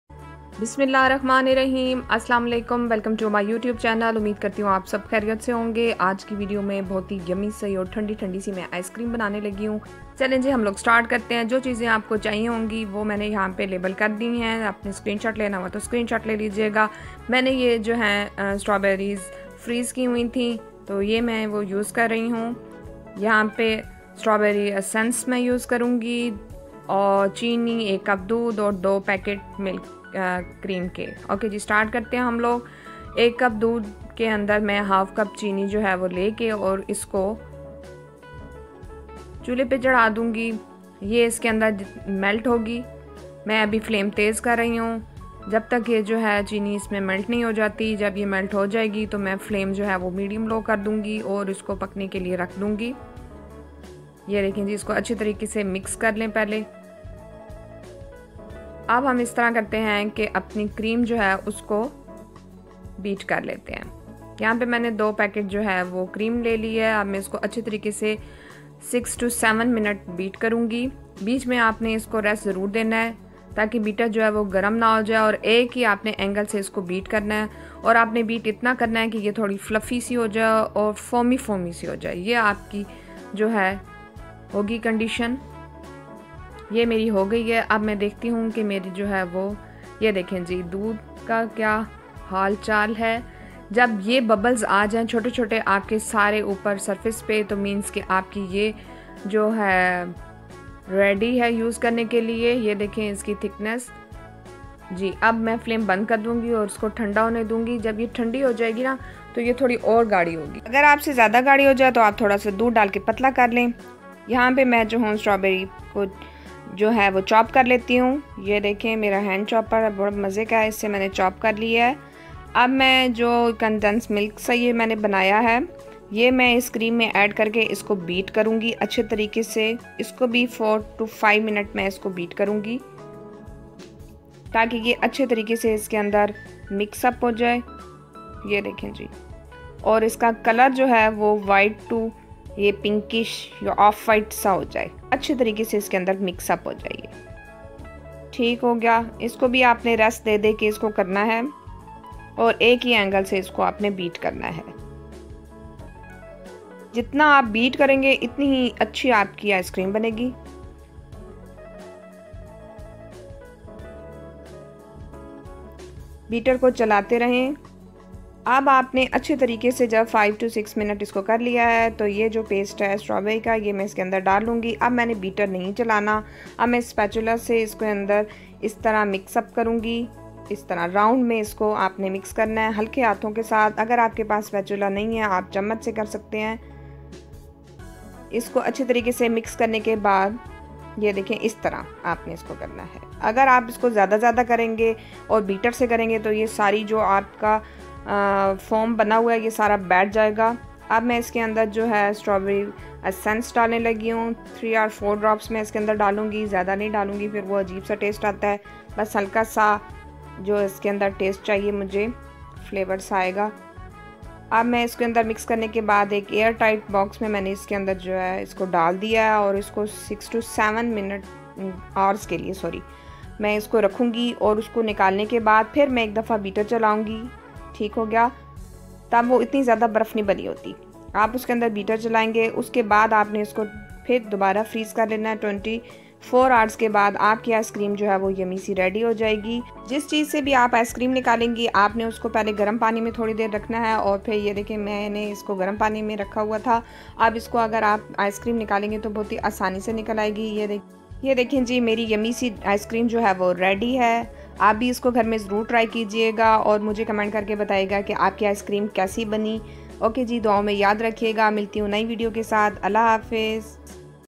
Easy & Rich Homemade Strawberry 🍓 🍨Ice-cream !!With only 3 Ingredients
Homemade Icecream Recipe